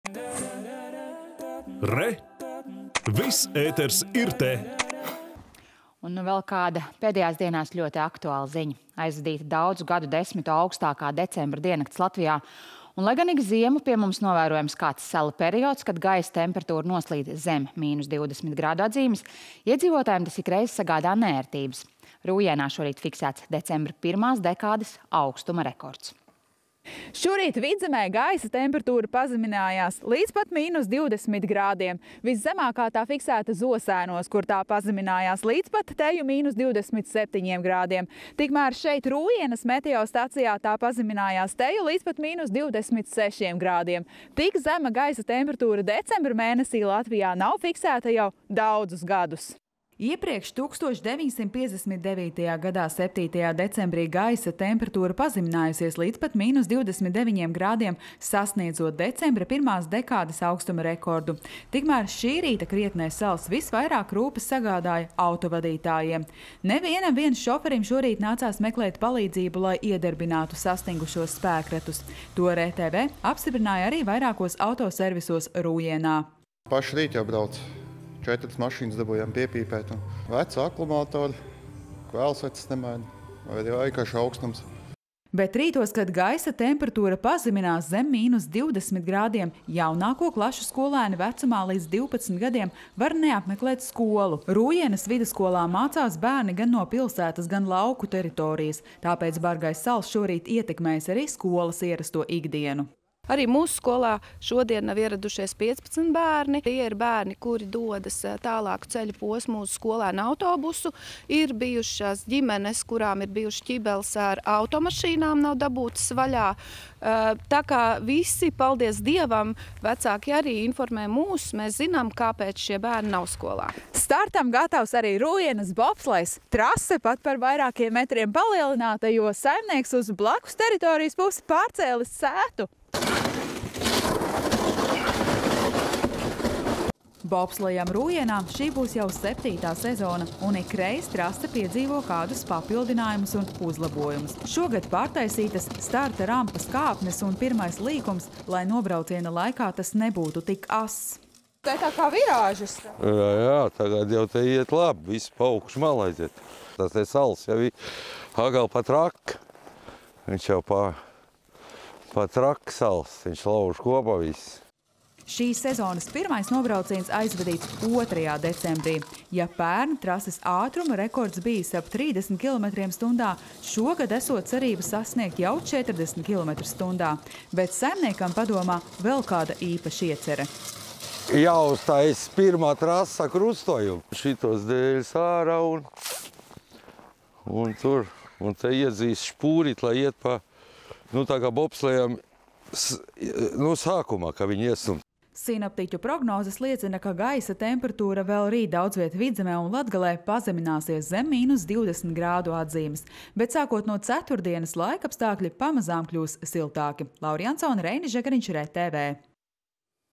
Pārspēts decembra pirmās dekādes aukstuma rekords
Aizvadīta daudzu gadu desmitu aukstākā decembra diennakts Latvijā. Un, lai gan ik ziemu pie mums novērojams kāds sala periods, kad gaisa temperatūra noslīd zem mīnus 20 grādu atzīmes, iedzīvotājiem tas ikreiz sagādā neērtības.  Rūjienā šorīt fiksēts decembra pirmās dekādes aukstuma rekords.